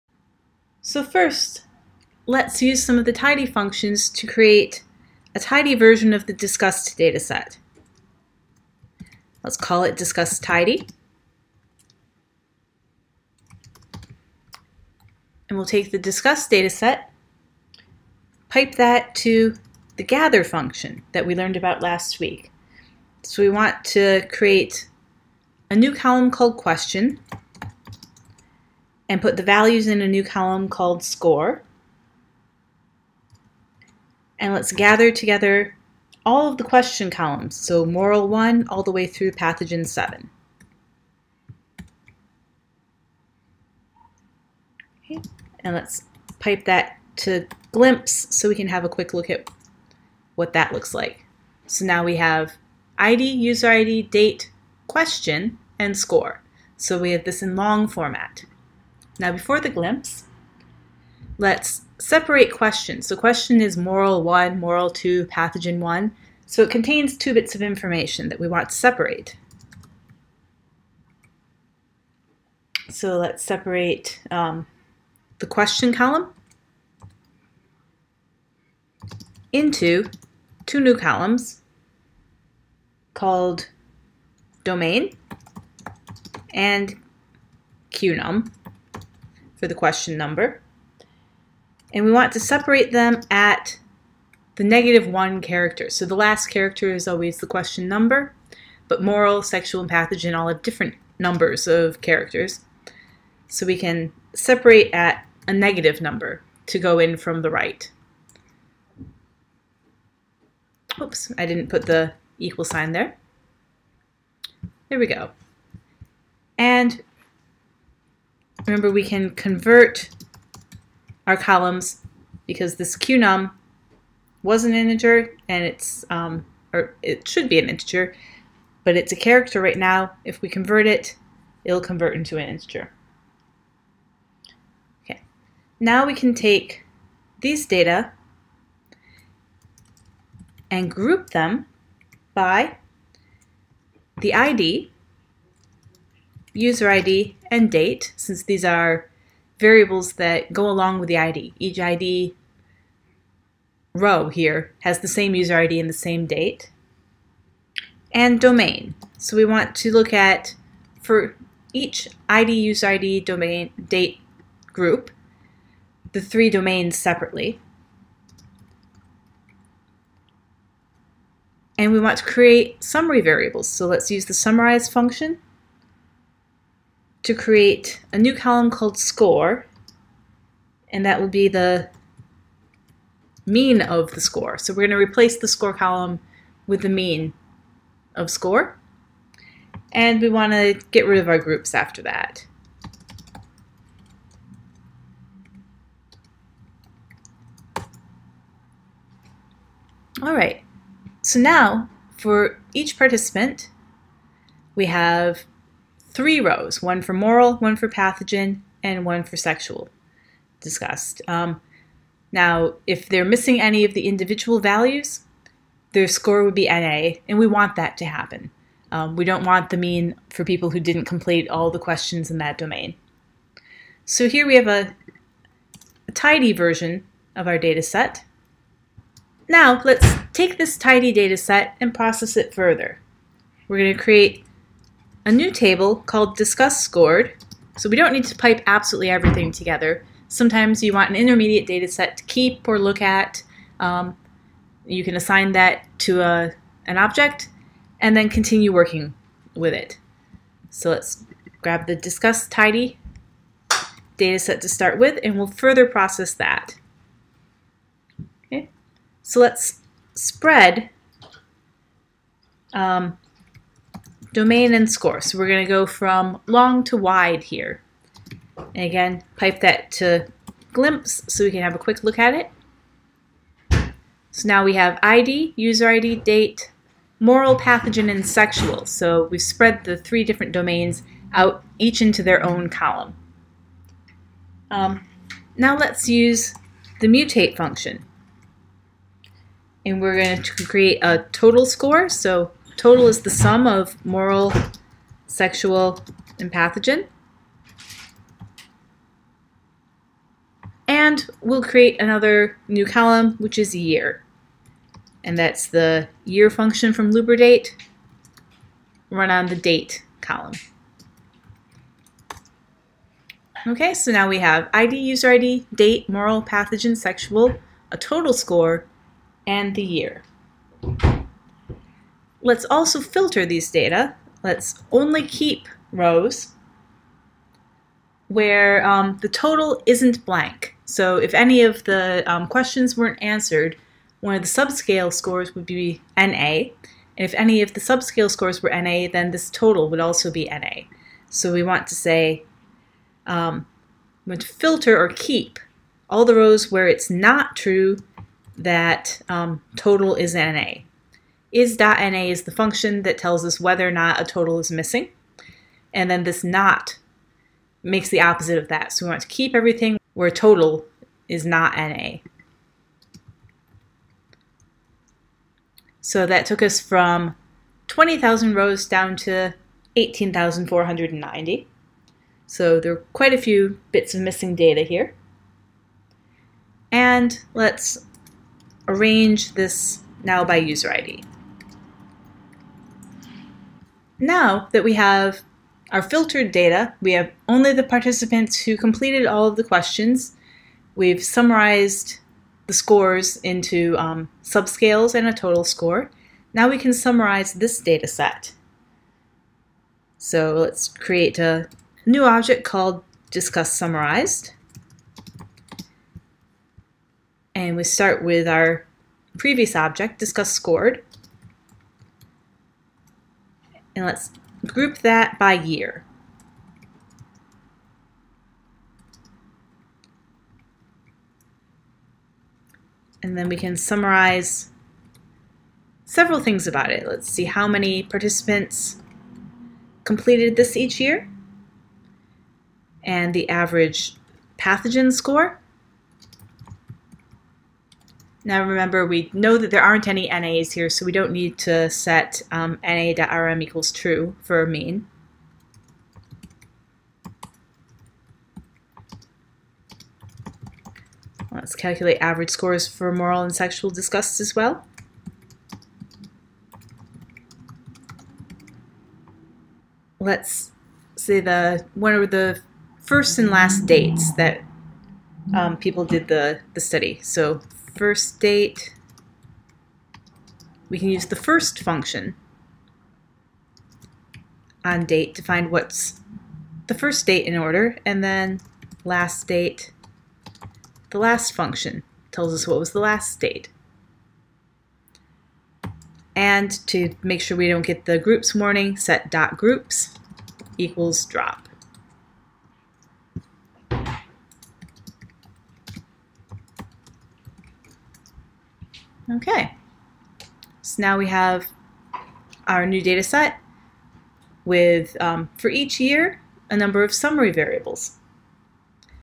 Chaining Functions
Data wrangling with tidyr and dplyr functions chained together with pipes